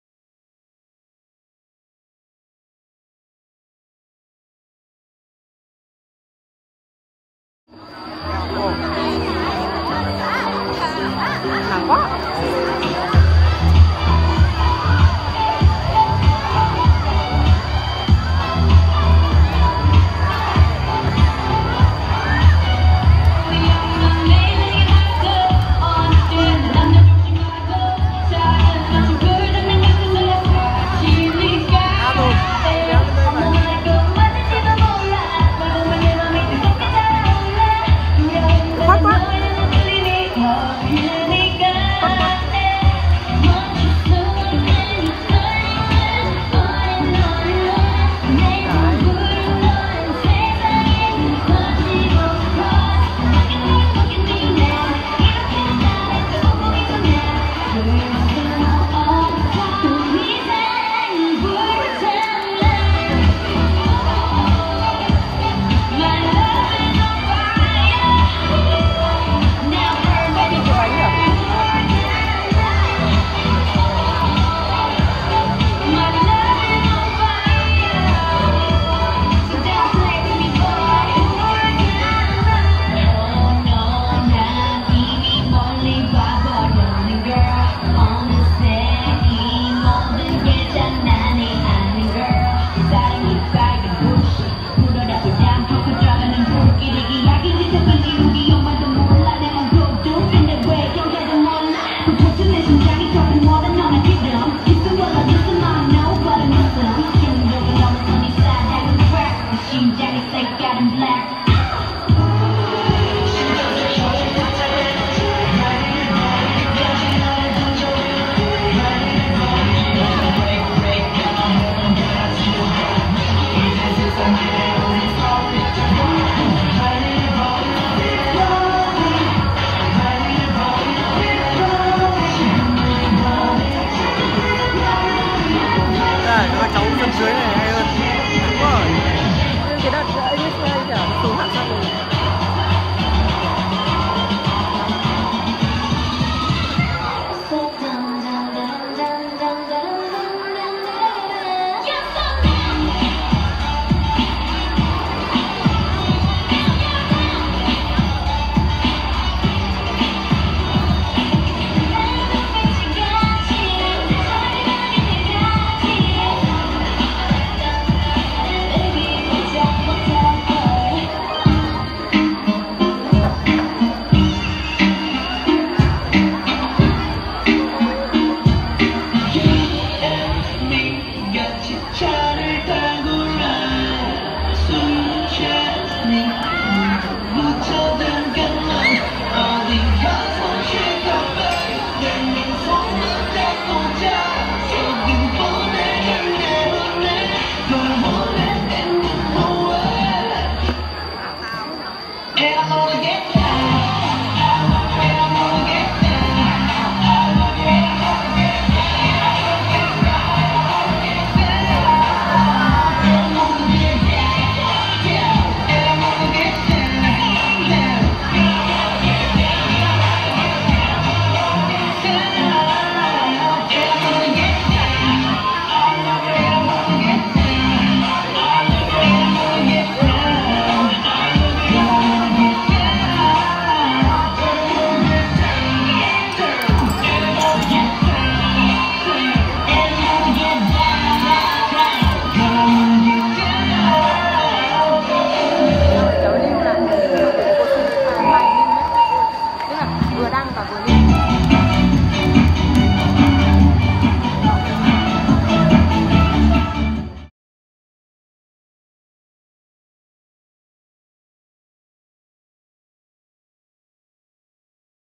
VĂN NGHỆ LỄ TỔNG KẾT - A2+A3 - K25
Phong trào Dance ở THCS Hạ Hoà ngày càng sôi động và các em cũng đang đạt đến level ngày càng cao.😍
Ko chịu kém cạnh các anh chị khoá trước, cũng rất nhiều bài free style đã được thực hiện. 😎😎